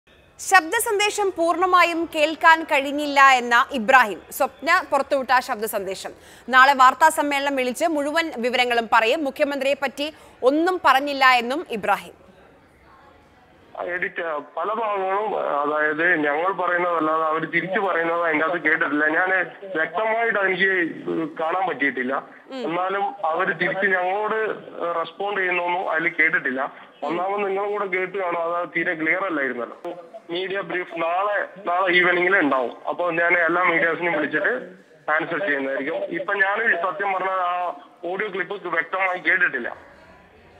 ശബ്ദസന്ദേശം പൂർണമായും കേൾക്കാൻ കഴിഞ്ഞില്ലെന്ന് ഇബ്രാഹിം | Mathrubhumi News
നാളെ വാർത്താസമ്മേളനം വിളിച്ച് മുഴുവൻ വിവരങ്ങളും പറയും. മുഖ്യമന്ത്രിയെ പറ്റി ഒന്നും പറഞ്ഞില്ലെന്നും ഇബ്രാഹിം.

Happy viewing!